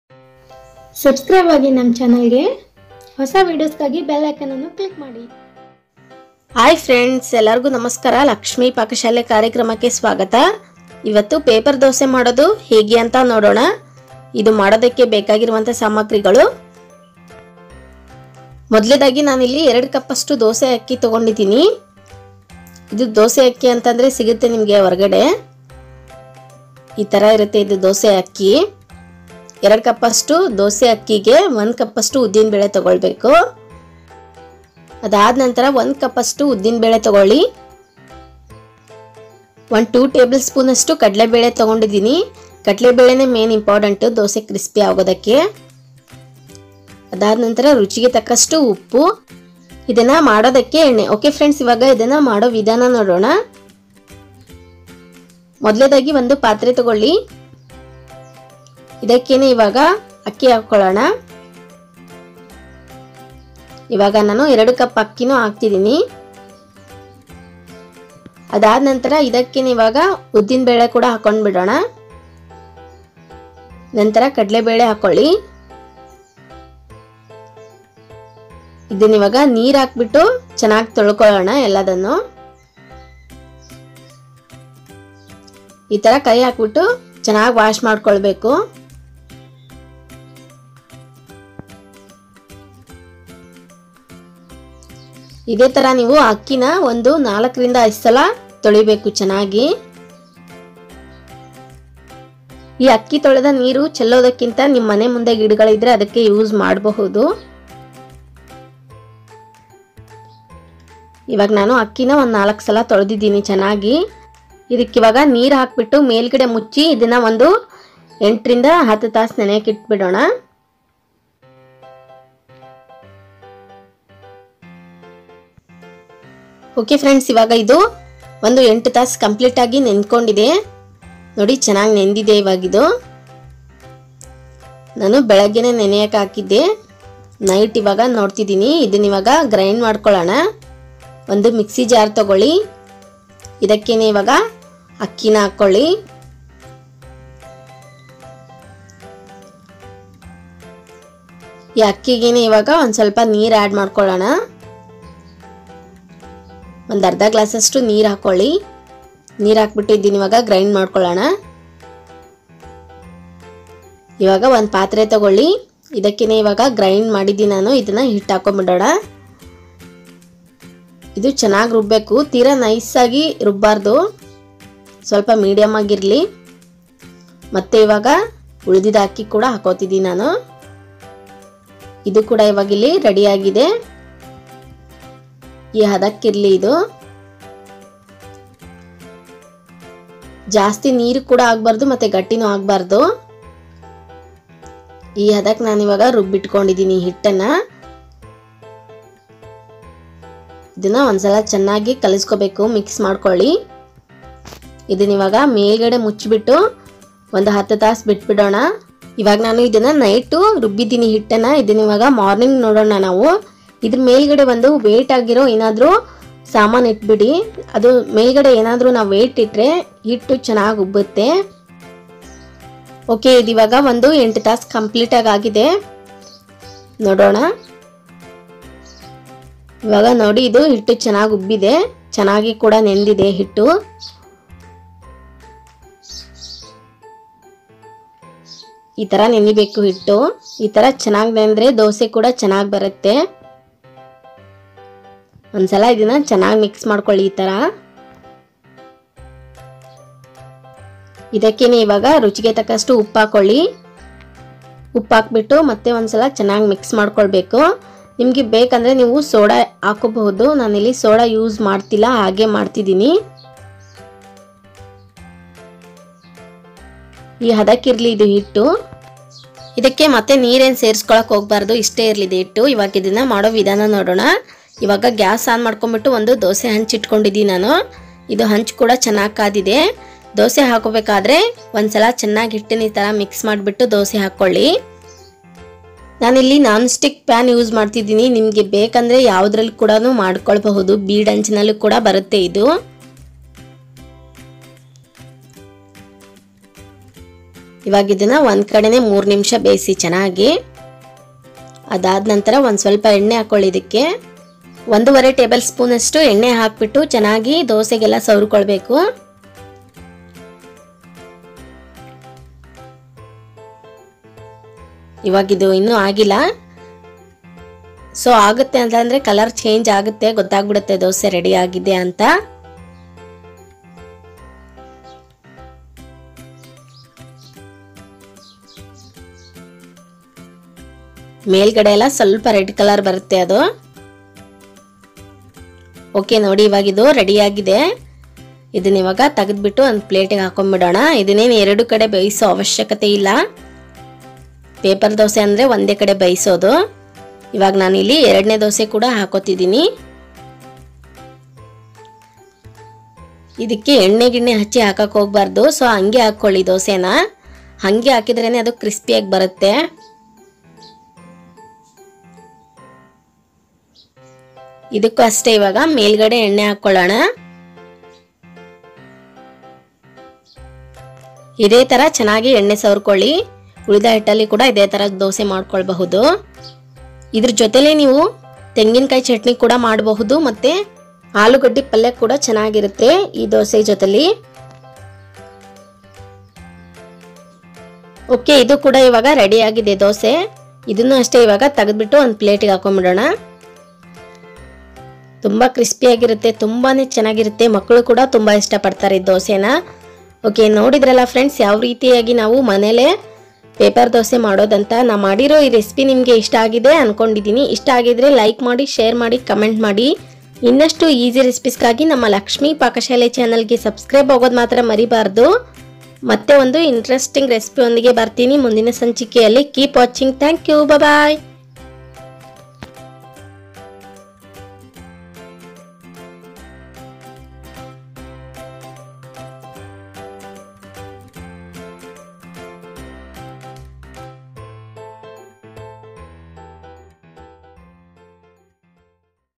ಪೇಪರ್ ದೋಸೆ | paper dosa recipe in Kannada | hotel style paper dosa super breakfast recipe
paper dosa

ingredients

dos rice- 2 cup 1/2 kg
urda dal- 1 cup 125gram
channa dal- 2 tbsp

for more recipes please subsribe my channel